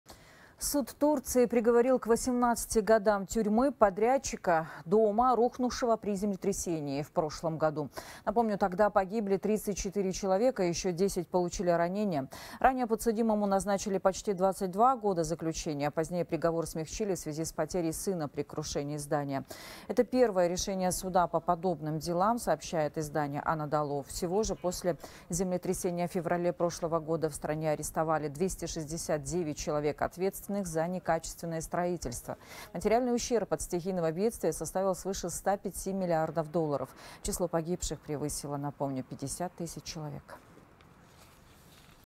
18 лет тюрьмы получил подрядчик рухнувшего при землетрясении дома
Суд Турции приговорил к 18 годам тюрьмы подрядчика дома, рухнувшего при землетрясении в прошлом году, передает «24KZ». Напомним, тогда погибли 34 человека и ещё 10 получили ранения.